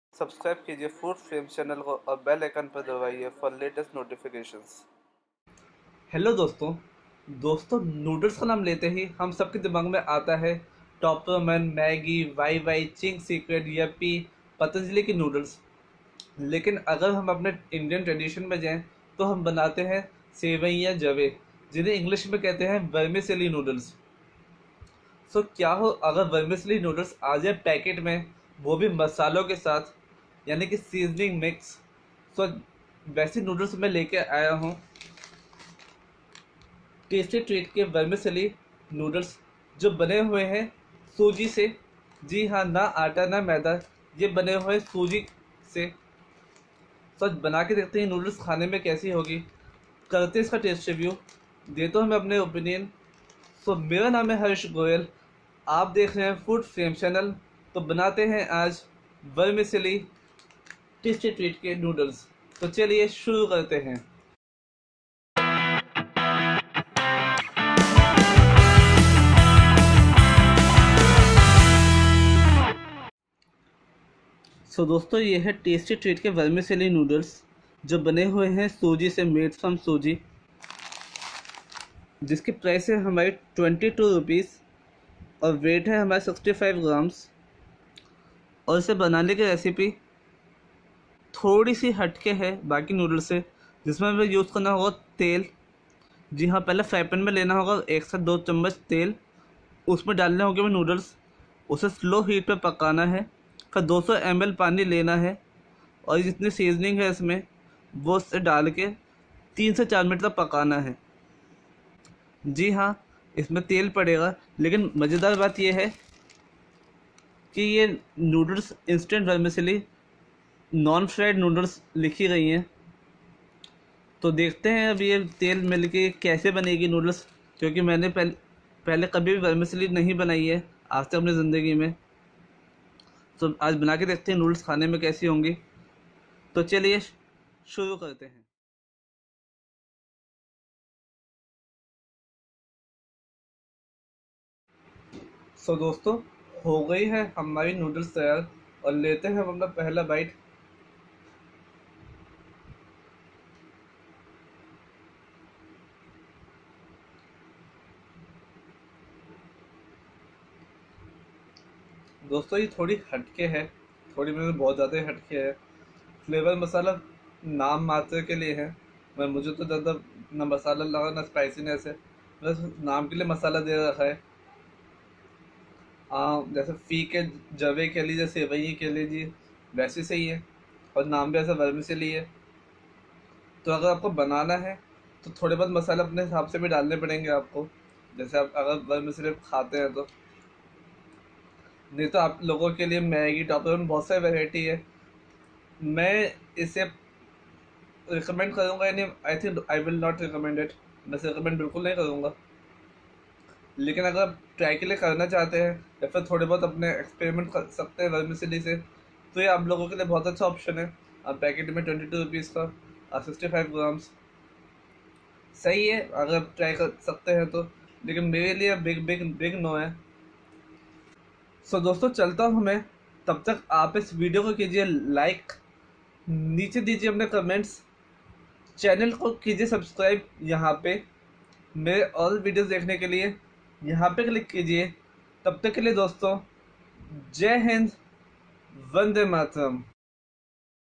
Tasty Treat Vermicelli Noodles- Taste and Review| Vermicelli with Seasoning Mix Masala.(नमकीन सेवई)
Hello Dosto, aaj mene try kiya hai Tasty Treat ke Vermicelli Noodles. Vermicelli jisey hum aam bhasha me नमकीन सेवई, नमकीन जवे bhi kehte hai.
Pr ye Vermicelli Noodles thi Seasoning Mix ke saath.
Iska Price hai, Rs 22/- and Weight hai 65g.

So kaisi lagi mujhe ye khaane me yahi mene btaya hai Video me.

Agar aapko Video Pasand Aayi toh Jarur Kijiiye Like 👍🏻
Niche Dijiye Comments